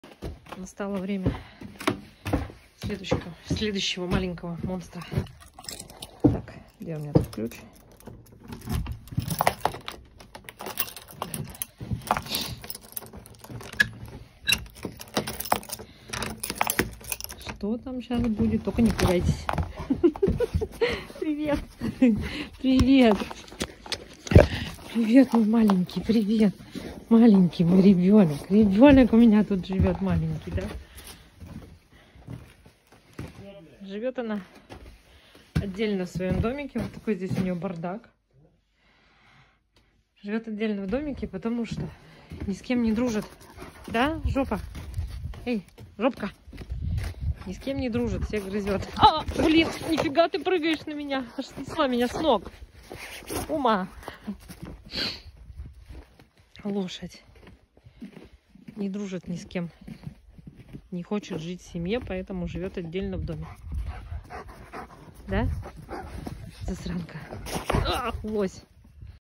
Алабай-злюка
Нашей малышке САО всего годик, но она уже очень характерная. Изначально наши девочки САО жили втроем в одном вольере, но месяц назад малышка Ума начала отстаивать свое лидерство, пришлось ее срочно отсадить, но на данный момент возможности построить новый вольер нет, поэтому мы временно определили ее в старый дом на нашем участке